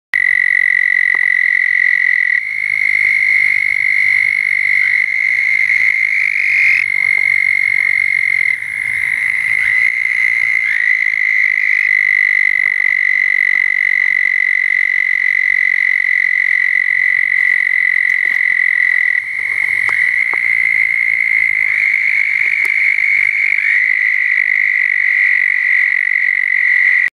Southern Toads Calling
First breeding night of the year - 2/22/2015. Found calling from vegetation along a pond in Tampa.